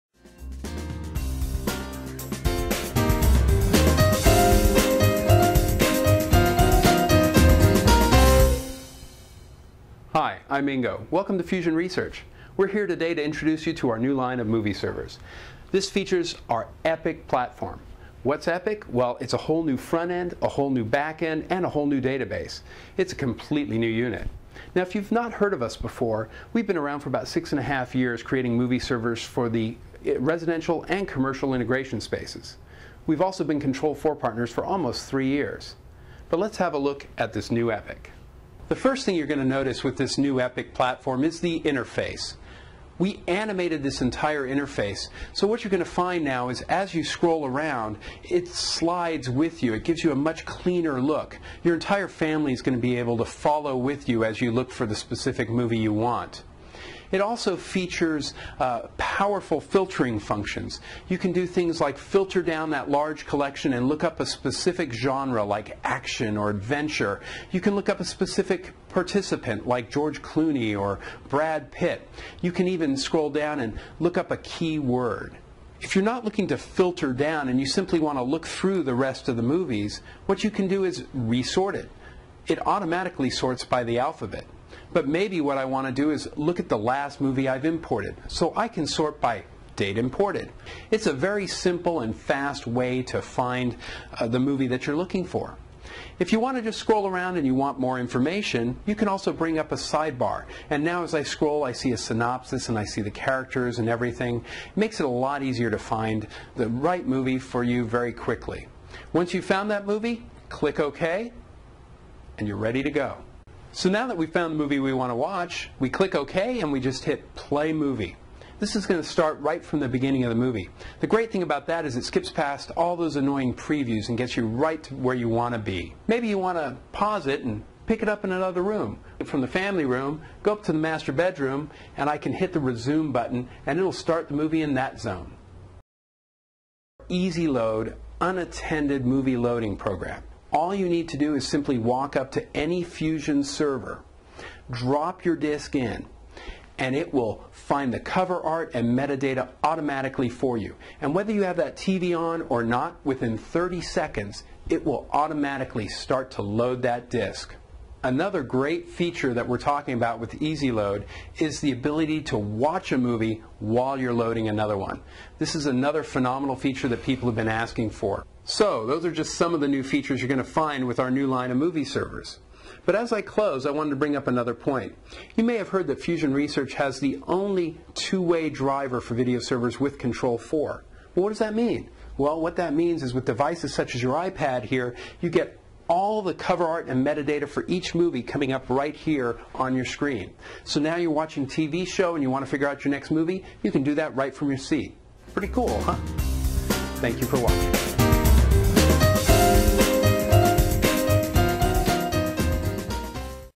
Fusion Movie Servers
Introducing Fusion's new line of EPIC Movie Servers for Control4